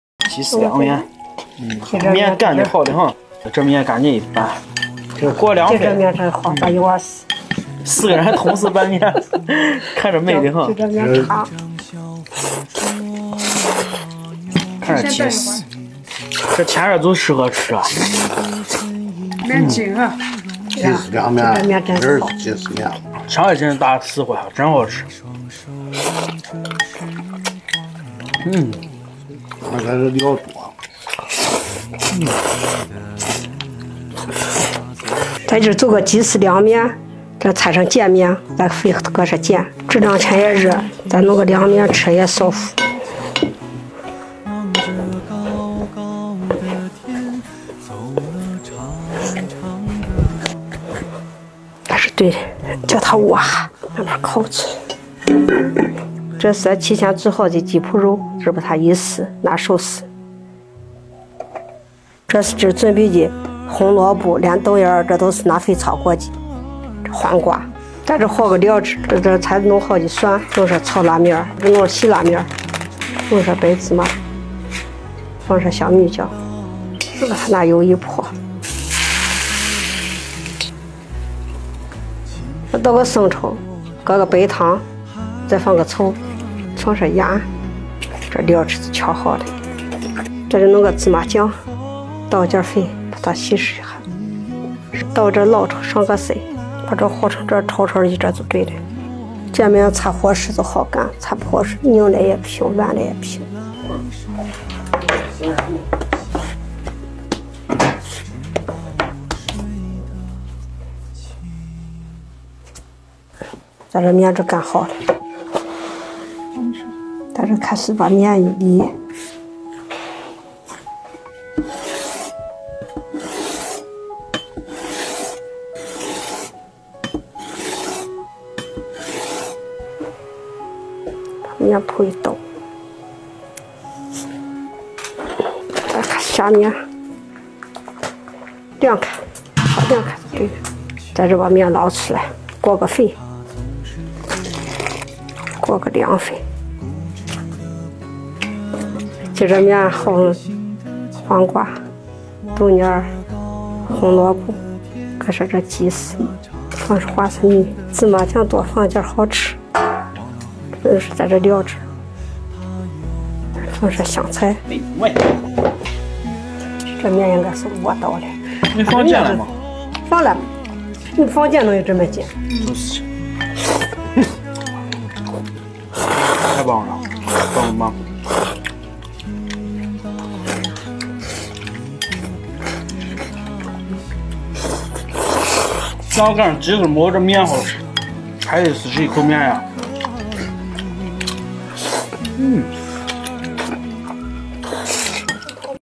夏天一到，就想来上一碗清爽的“鸡丝凉面”
大家好，这里是【乡村萍婶】官方频道
蓝田勺勺客/农村大厨☘️
红白喜事流水席从厨三十余年
分享家乡美食、记录农村生活
欢迎评论区点菜